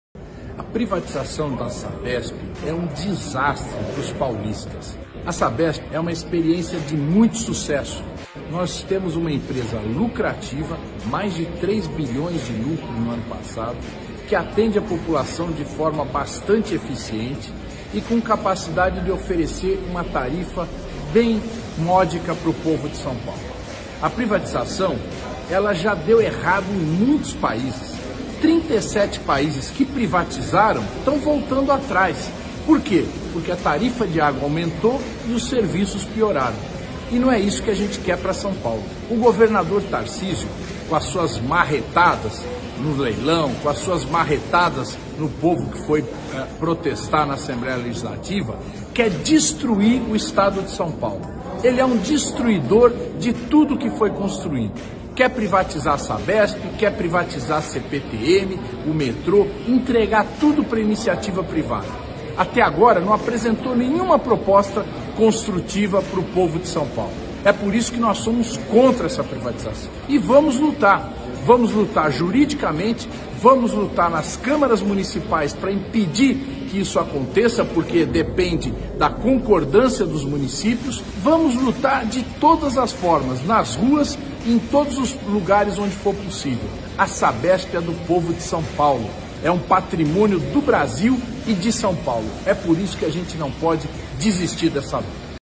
ACORDA, São Paulo! 📢
Precisamos lutar contra a privatização da Sabesp! O governador Tarcísio está enganando as pessoas!
Se venderem a Sabesp a tarifa vai ficar mais cara e o serviço vai piorar.